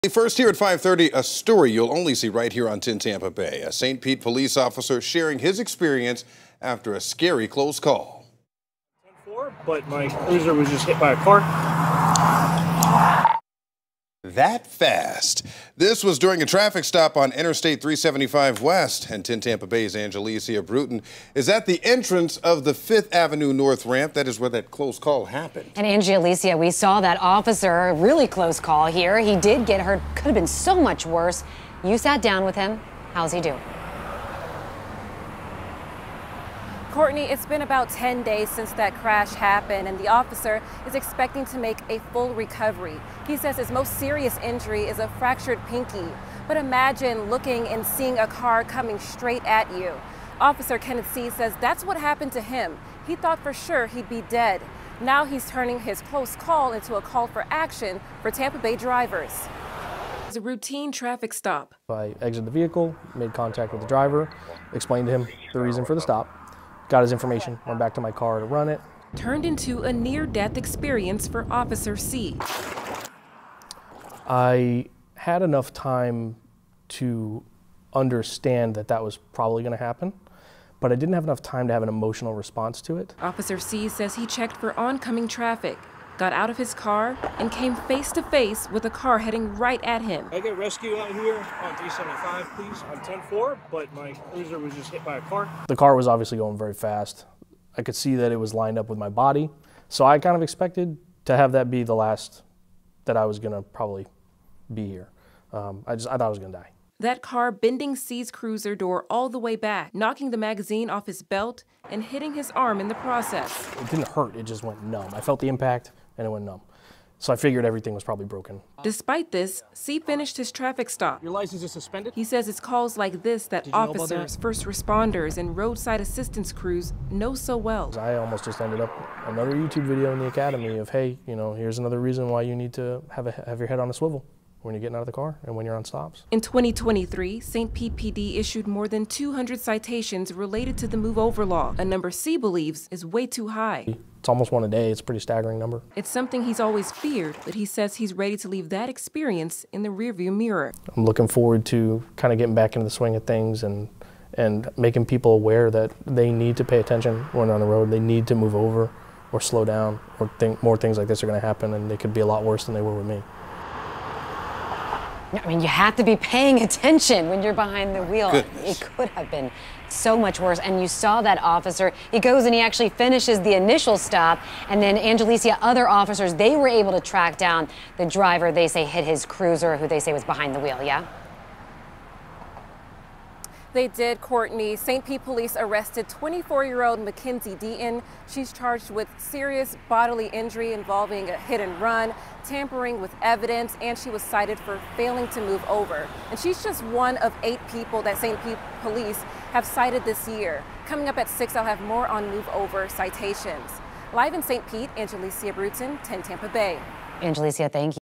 St. Pete police officer speaks out after close call with driver who didn't move over
A St. Pete police officer is speaking out after a close call with a driver who didn't move over.